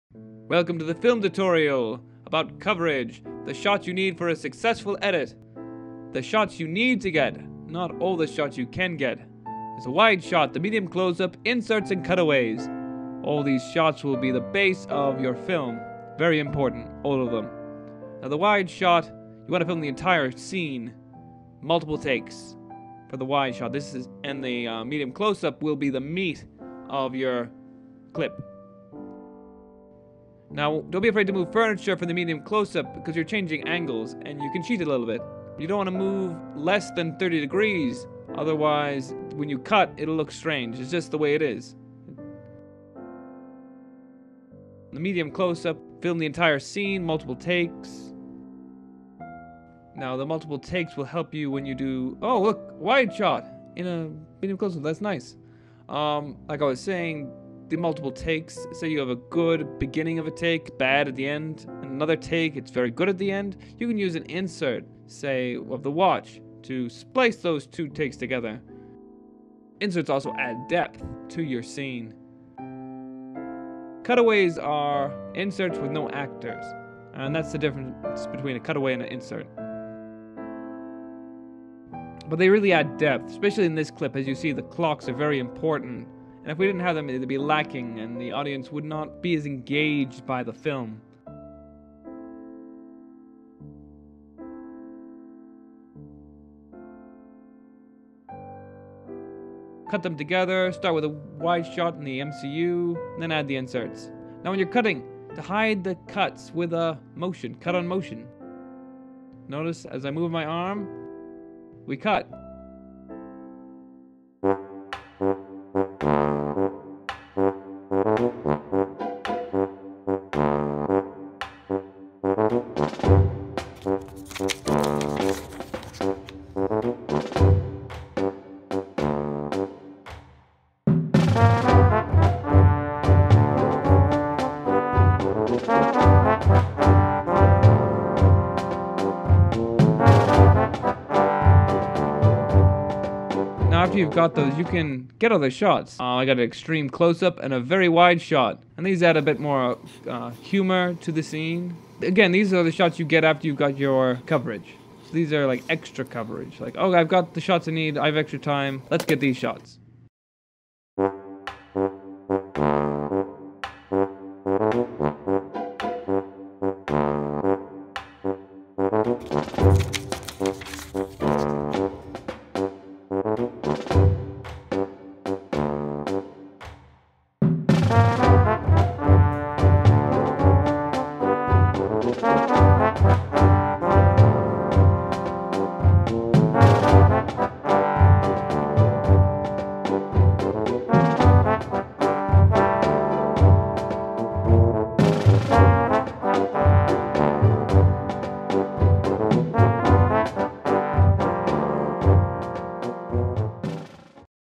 Basic Film Coverage: One Subject
The scene took me forty minutes to film and forty minutes to edit. I used an  iPhone 6s and iMovie for the clip. The rest was done in Premiere Pro.